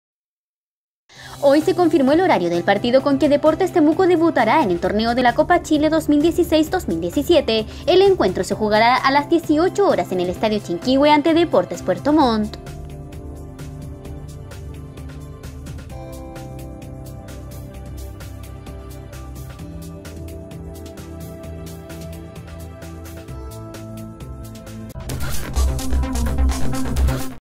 Se confirmó el horario para el debut de Deportes Temuco en la Copa Chile
Los albiverdes jugarán este sábado ante Puerto Montt en el Estadio Chinquihue. El partido se disputará a las 18:00 horas.